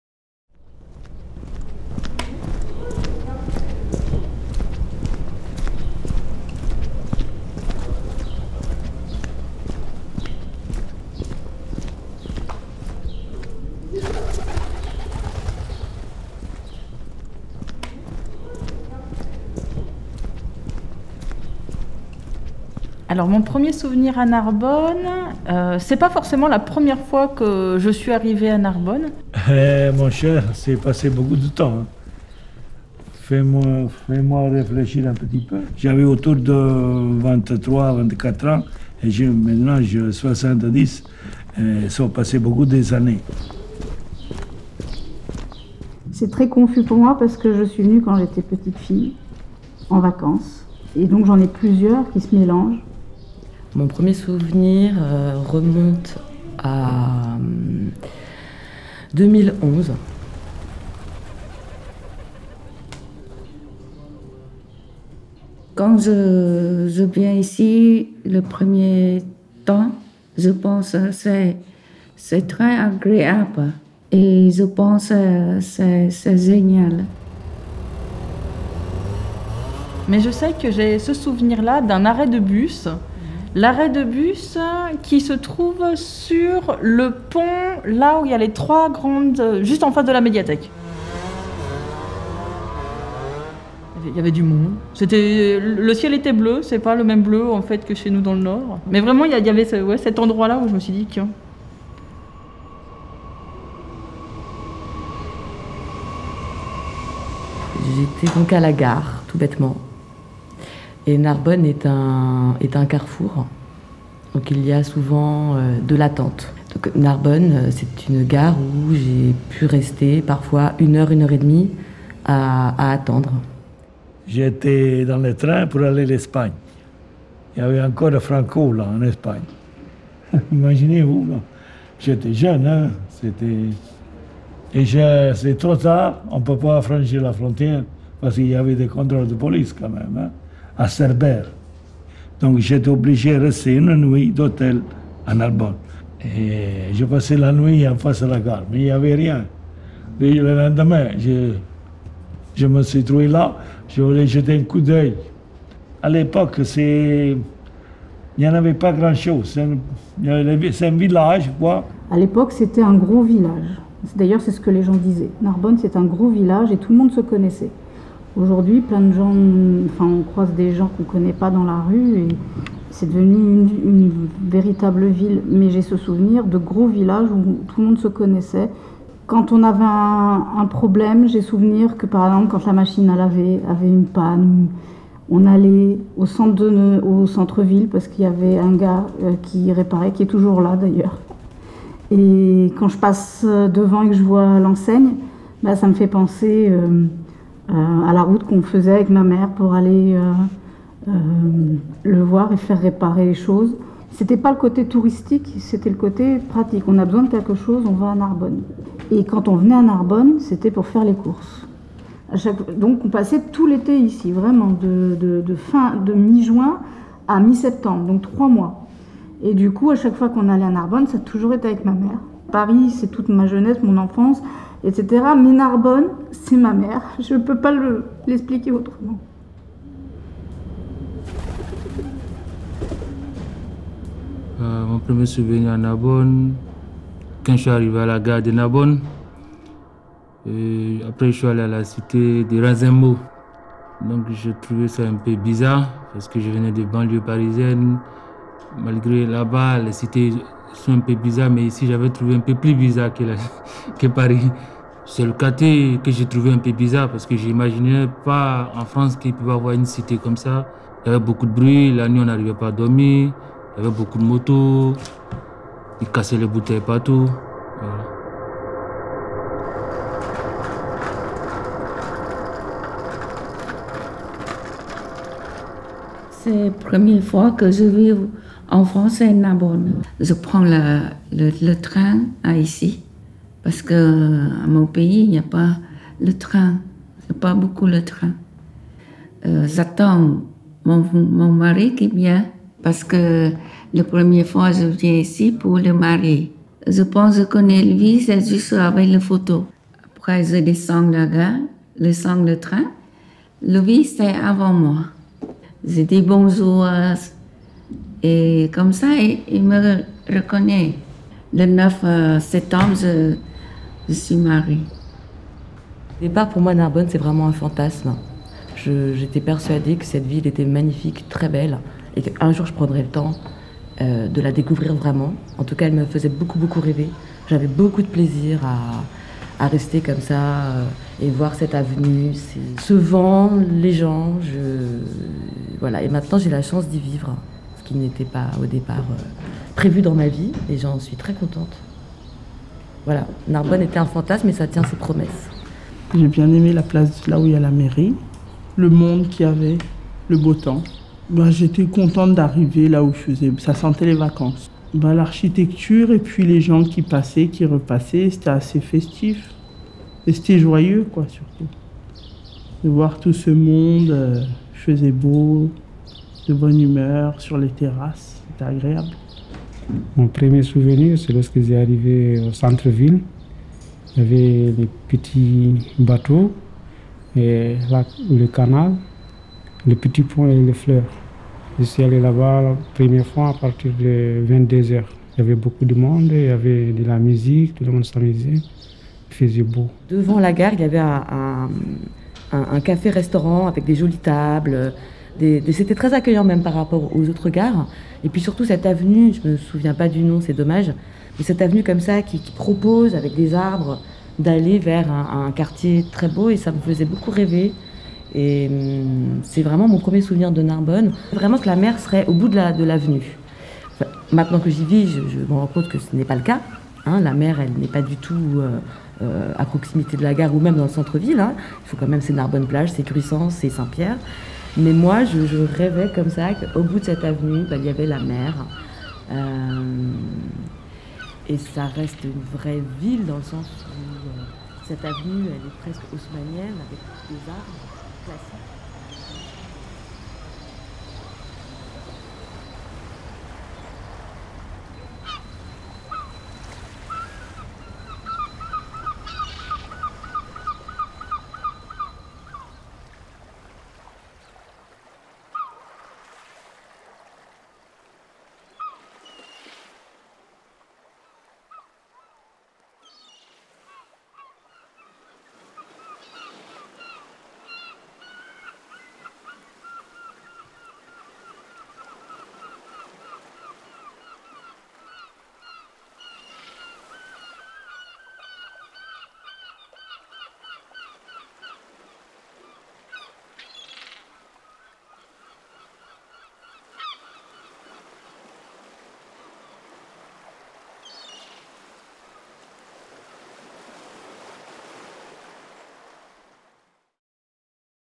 Souvenirs Narbonnais
Le projet est né de l’envie d’allier souvenirs, sons et images…
Le premier souvenir est propre à chacun⋅e et est aussi universel. Lorsqu’on arrive quelque part, se grave dans nos mémoires ce premier souvenir, souvent lié à une impression, qui nous donne envie d’explorer, de s’installer, de partir... ou qui nous questionne.
C’est ce premier vécu de la ville, Narbonne, que nous souhaitons ici mettre en lumière dans ce projet. Par le son, avec la réalisation de deux créations sonores qui évoquent ces souvenirs, par l’image avec cette exposition photographique qui évoquent le temps qui passe.
Le projet s’est déroulé de mars à juin 2021 au Forum.

Les participant⋅e⋅s :
Babé, Alpha, Yacouba, Mario, Fleur, Siga, Thira, Fabienne (stagiaire), Fabienne et Inès pour les témoignages et la réalisation des photos.

Projet financé par l'ANCT, la CAF, le Grand Narbonne et la ville de Narbonne, coordonnée par la maison des Potes, conçu et réalisé par KOVisuel.